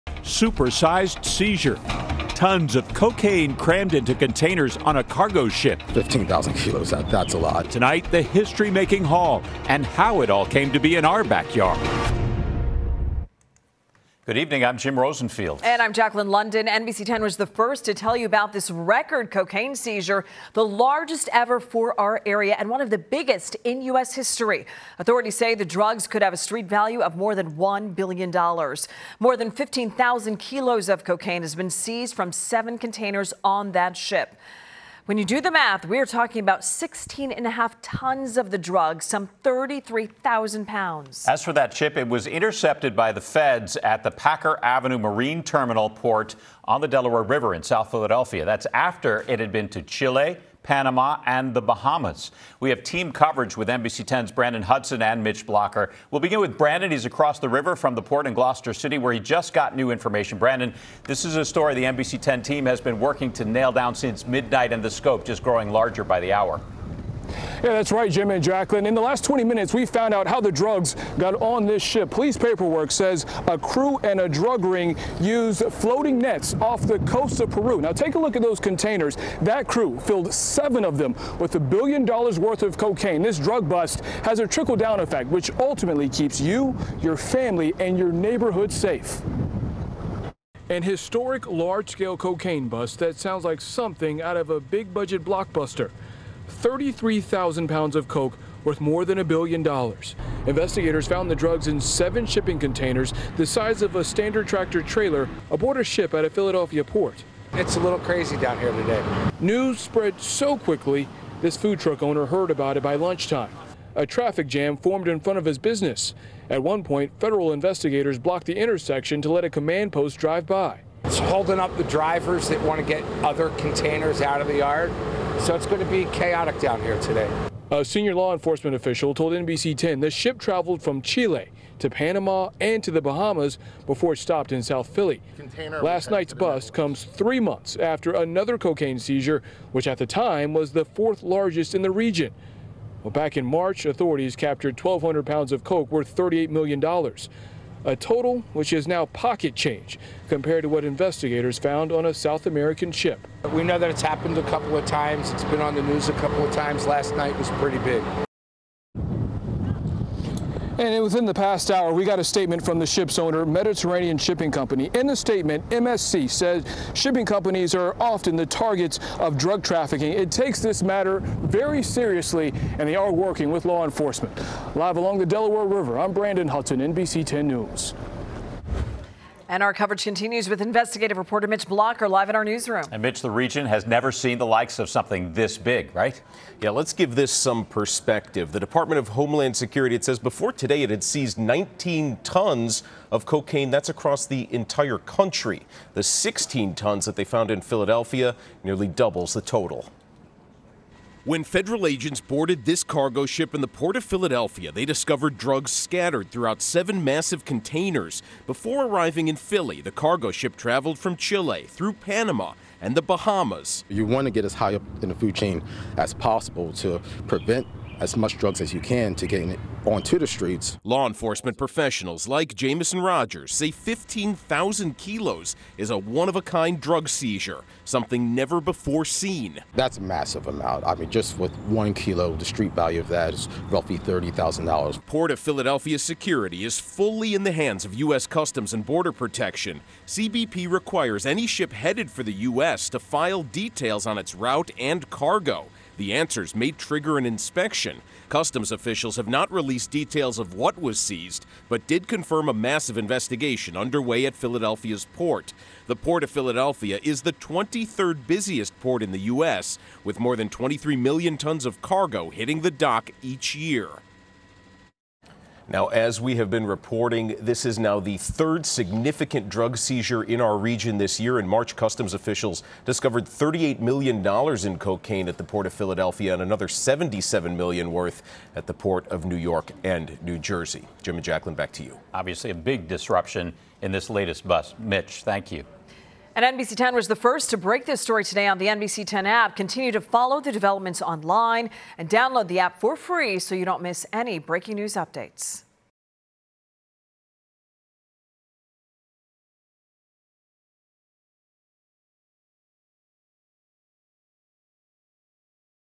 $1 Billion in Cocaine Seized at Philly Port, Crew Charged | NBC10 Philadelphia
Shipping containers full of illegal drugs have been found and seized by federal authorities at a Philadelphia port in what federal authorities describe as the largest seizure in the region's history.

A senior law enforcement official said more than 15,500 kilos of cocaine were found in seven containers, which were found aboard a cargo ship, the MSC Gayane, that previously stopped in Colombia, Chile, Panama and the Bahamas.

At least two crew members of the Gayane have been charged with trafficking, according to a complaint filed Tuesday by the U.S. Attorney's Office. Other crew members were also allegedly involved, the complaint charges.

The bust occurred at the Packer Avenue Marine Terminal Port on the Delaware River in South Philadelphia, multiple local, state and federal law enforcement sources have told NBC10.